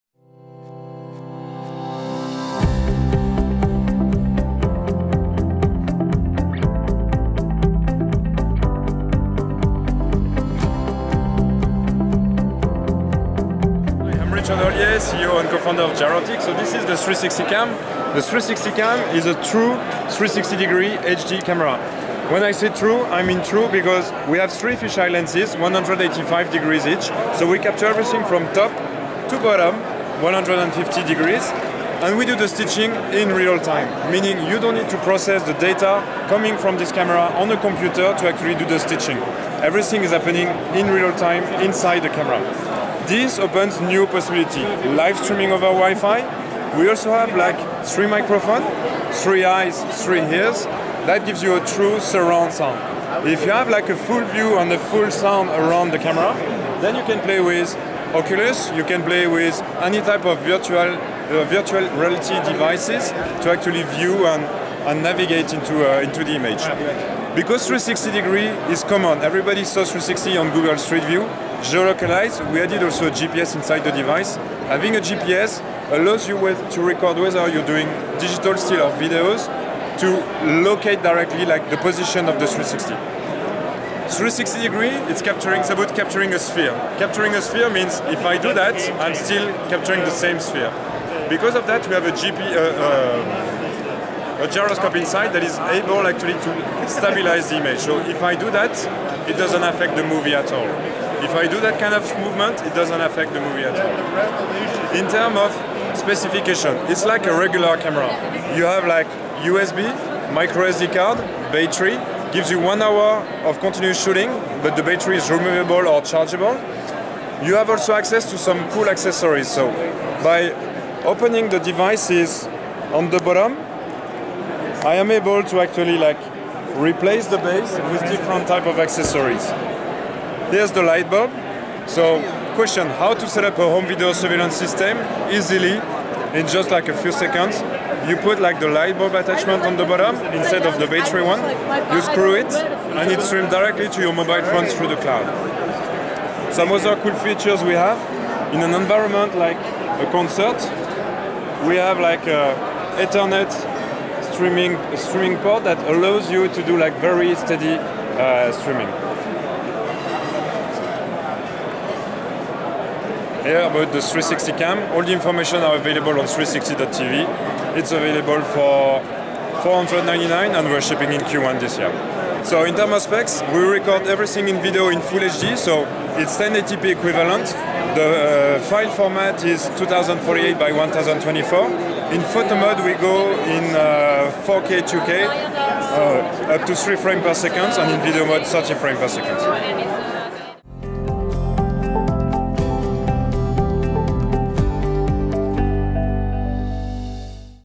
Giroptic 360 Cam - CES 2015
True 360 degree camera, realtime stitching, 3 lenses, 3 microphone, multimount, hardware CAT, or wireless, GPS, stabilizer, removable battery, microsd, $499-, available shortly, 1080, HD